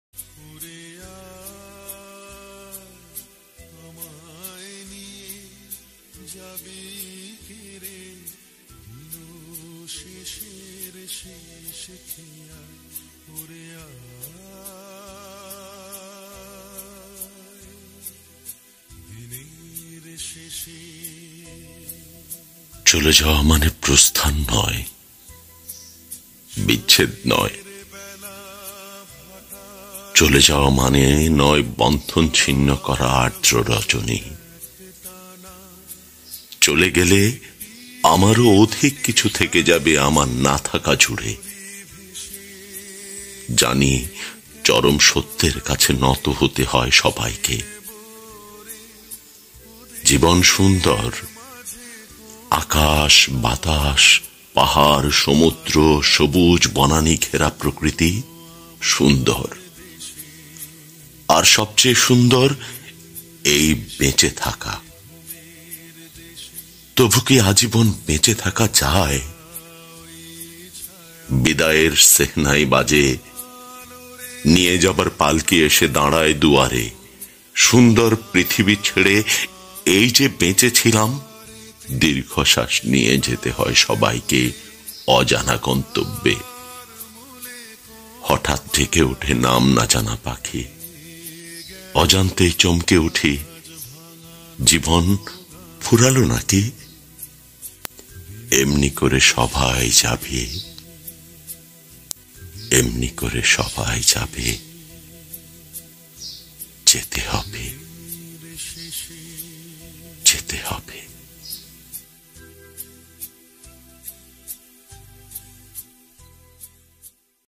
রুদ্র মুহম্মদ শহিদুল্লাহর চলে যাওয়া মানে প্রস্থান নয় আবৃত্তি বিদ্যুৎ বিহারি Biddut Behari
রুদ্র মুহম্মদ শহিদুল্লাহর "চলে যাওয়া মানে প্রস্থান নয়", আবৃত্তি: বিদ্যুৎ বিহারি Poet: Rudra Mohammad shahidullah, Recitation: Biddut Behari famous Bangla poem শ্রেষ্ঠ বাংলা কবিতা আবৃত্তি সময়ের চমকে যাওয়ার কবিতা করোনা বিষয়কে ছুঁয়ে যায় this poem represents coronavirus covid-19.